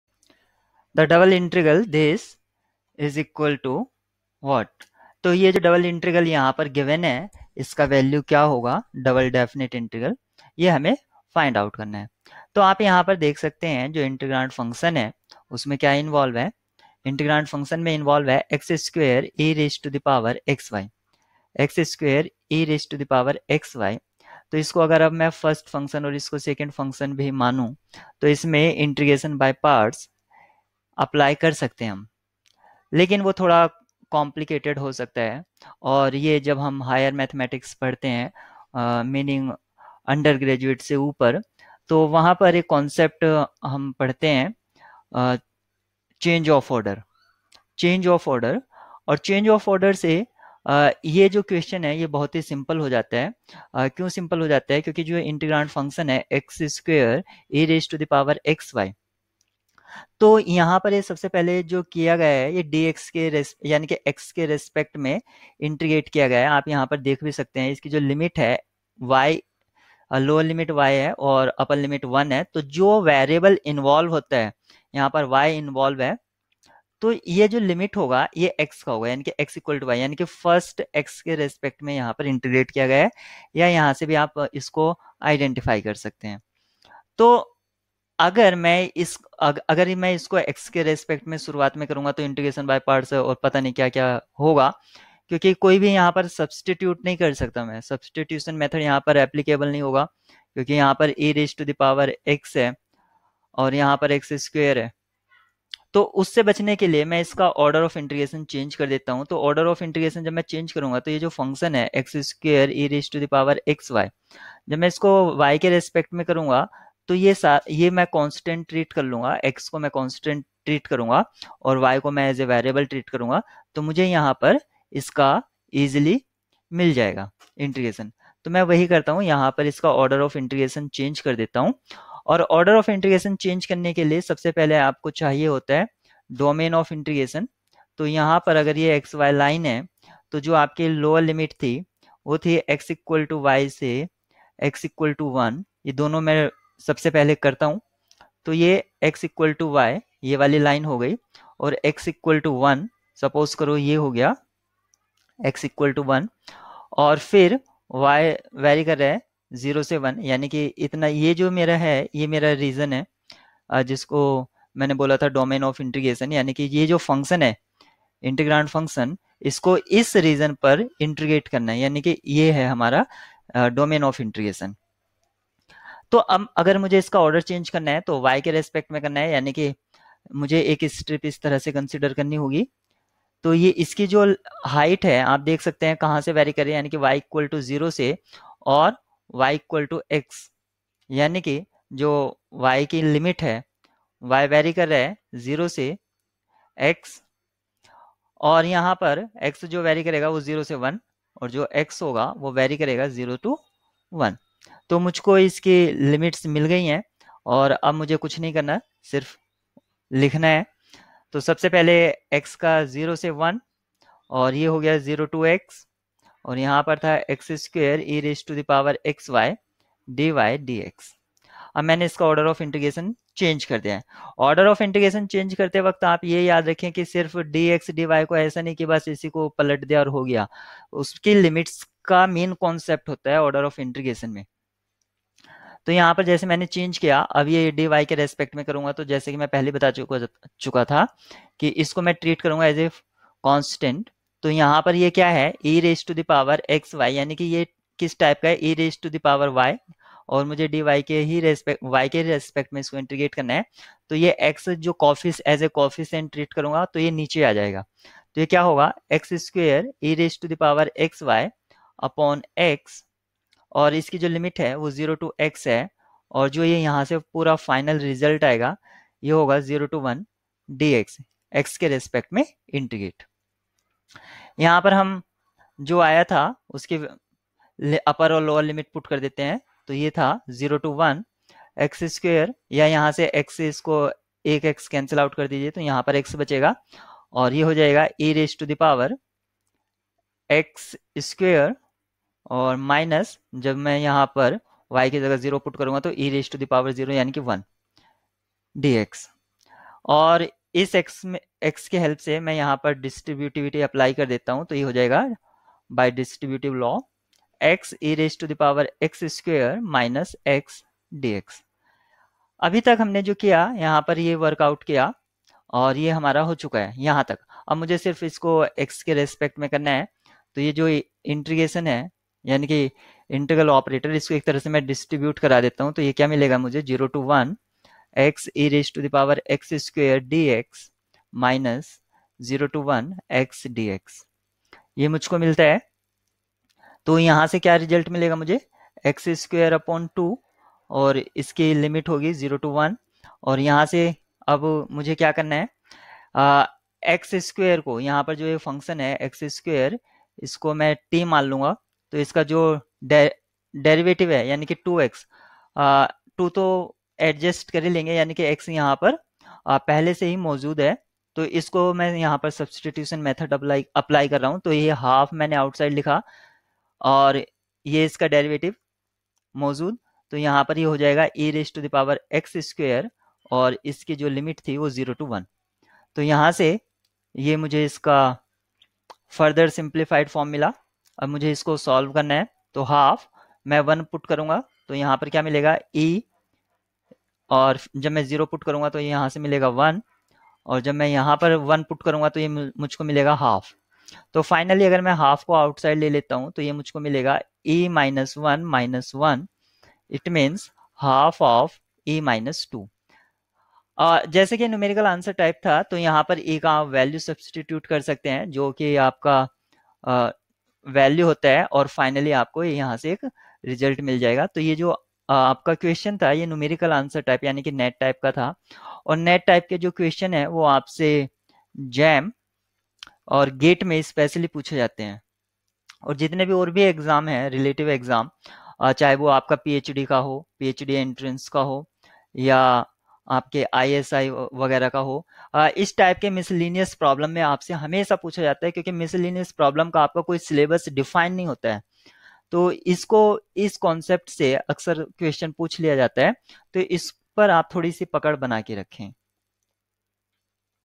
Change of Order of Integration for Gate Mathematics IIT Jam 2006 Mathematical Statistics
Syllabus of Integral Calculus for Jam mathematics: Integration as the inverse process of differentiation, Definite integrals and their properties, Fundamental theorem of integral calculus, Double and triple integrals, change of order of integration, Calculating surface areas and volumes using double integrals and applications, calculating volumes using triple integrals and applications.

Jam MS 2006, 27(a)
 1/2(e-2)